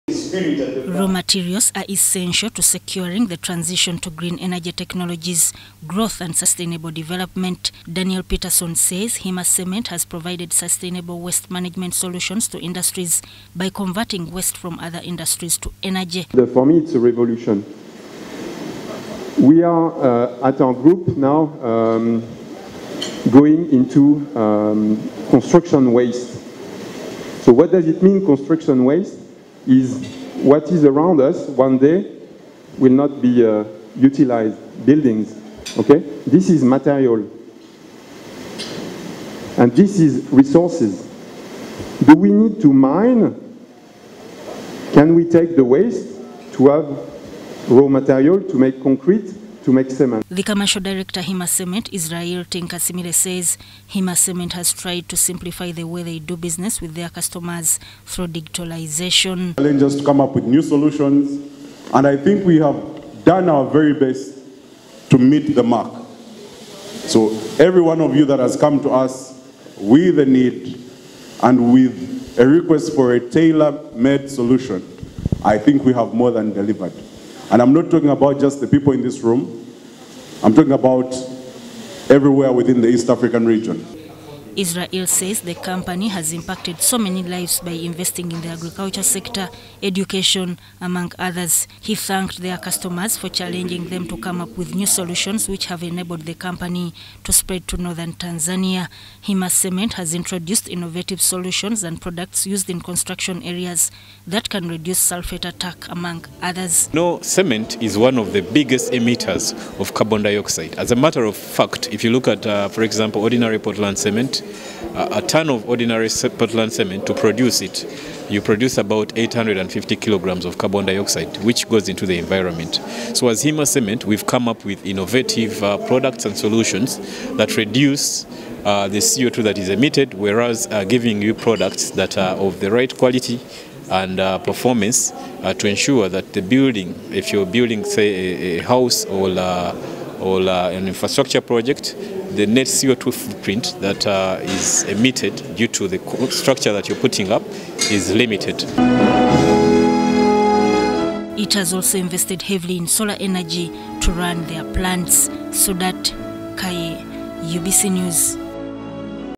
Hima cement to convert waste into raw material
Hima Cement has set a goal of taking a new path in waste management, making it a potential raw material.
The Hima Cement Country Chief Executive Officer Daniel Peterson says post-consumer waste, industrial scrap and unwanted by-products must be viewed as raw materials that are underused.
Peterson made the remarks in a meeting with Hima Cement Customers in Kampala.